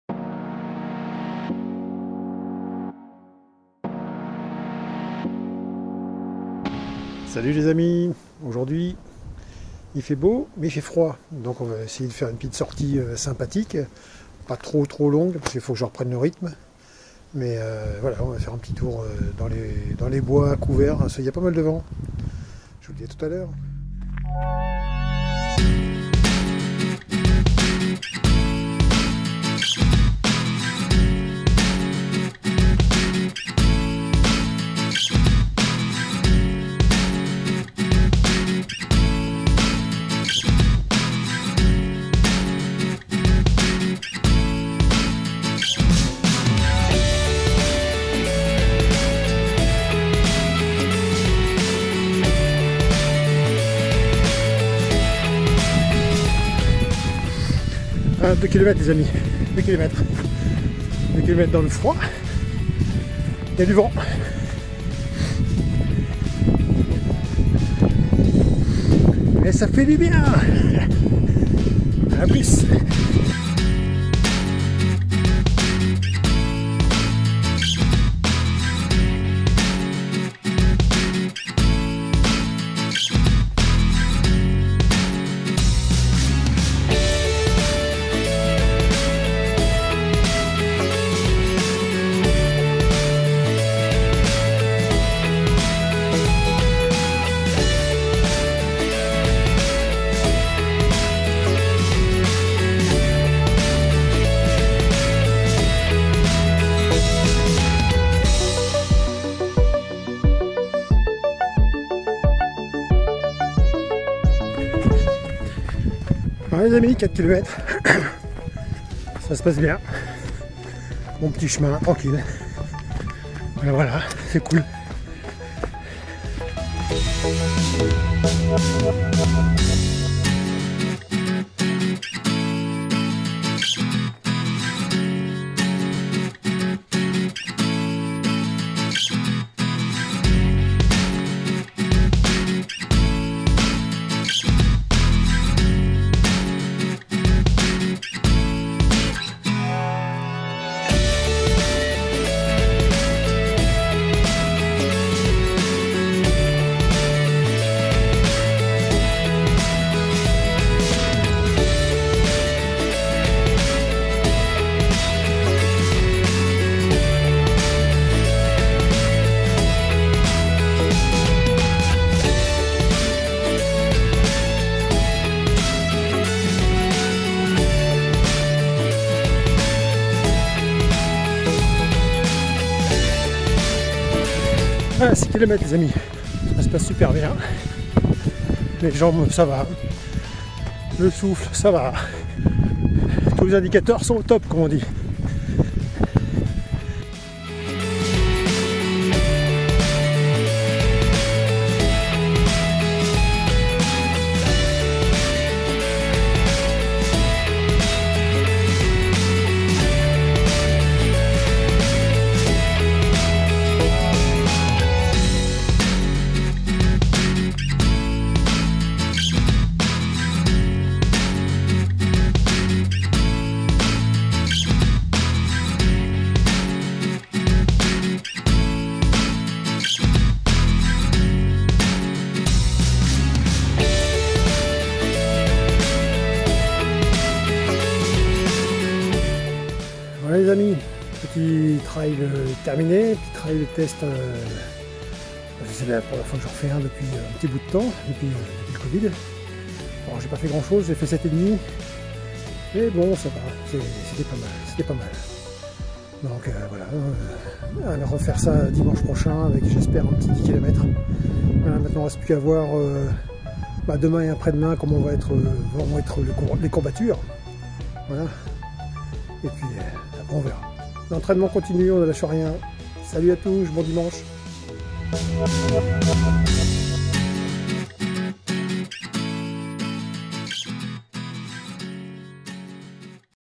Premier entrainement Trail de l'année. Après avoir eu le Covid, ce n'est pas simple de reprendre...
Mon premier entrainement Trail de l'année.